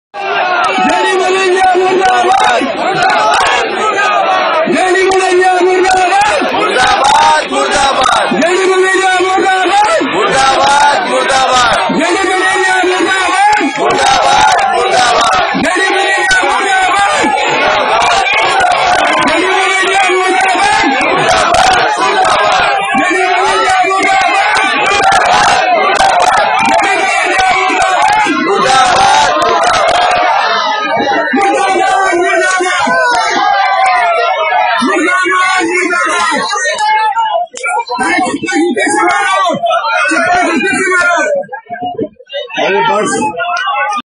FWICE strike gets nasty: Protesters organise funeral of J D Majethia (IFTPC head)
FWICE strike gets nasty...protesters organise funeral of IFTPC head J D Majethia...exclusive footage at IndianWikiMedia